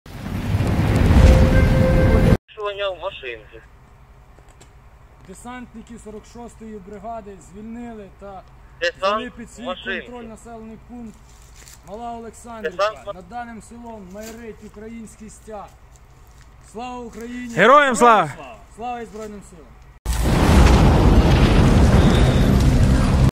Військові підняли прапор у ще одному селі на Херсонщині
4 жовтня підрозділи однієї з бригад Десантно-штурмових військ Збройних Сил України звільнили від російських окупантів населений пункт Мала Олександрівка на Херсонщині.